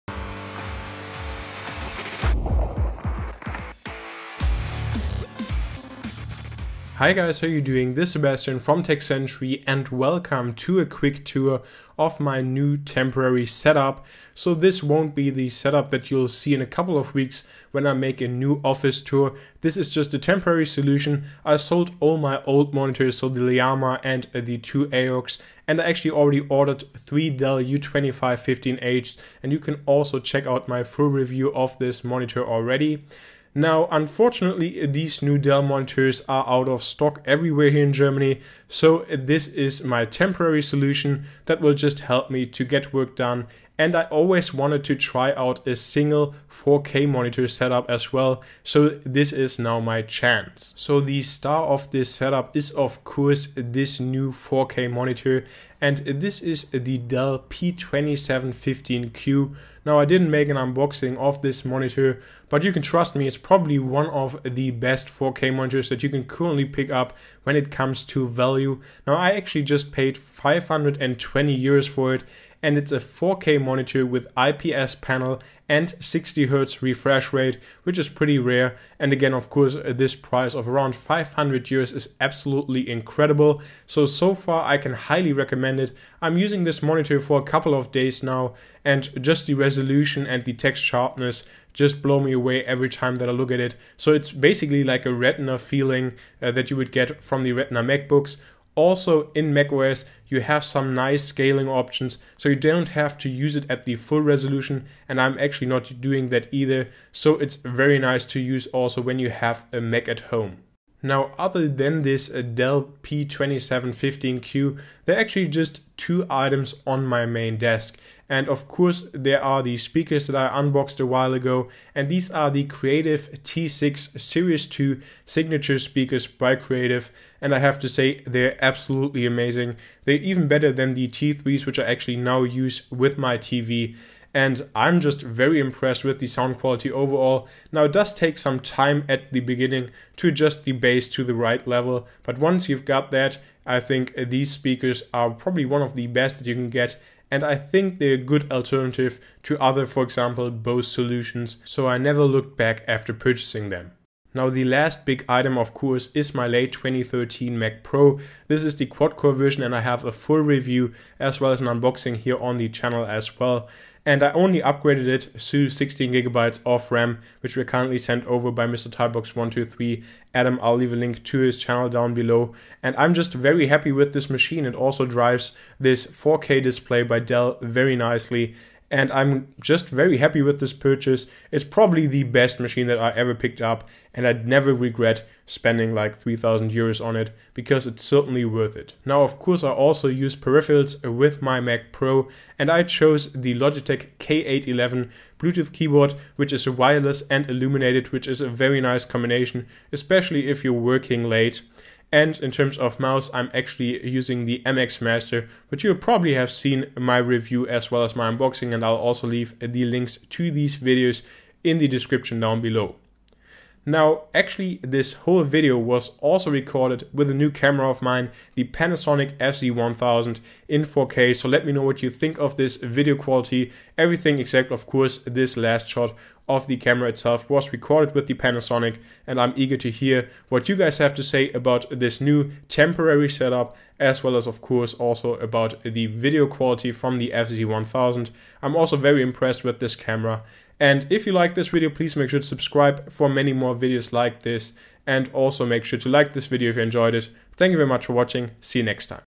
TechCentury Geek Desk Tour 2015 in 4K / Mac Pro, 4K Monitor, Logitech Master
Welcome to a tour of my 2015 desk setup. The setup that you see in this video is only a temporary solution while I wait for my 3 Dell U2515H monitors but I actually quite like its simplicity so I wanted to share it with you.

I am very impressed with my new monitor, the Dell P2715q. It is a 4K display with an IPS panel as well as a 60Hz refresh rate. For a price around 520€ it delivers stunning picture quality and a great text sharpness.

As my main computer I am still using the late 2013 Quad-Core Mac Pro by Apple with the Dual D300 GPUs. It is still a fantastic machine that is definitely worth the high asking price for me.

Items in this video:
- Dell P2715Q 4K IPS Monitor (60Hz)
- Creative T6 Series 2 Speakers
- Apple Mac Pro (late 2013)
- Logitech K811 Keyboard

Patrons who make this content possible:
Kerstin Eiwen
Alexandra Schneider
TheMacSector
Michael Weber
Alexander Münch
WebCreator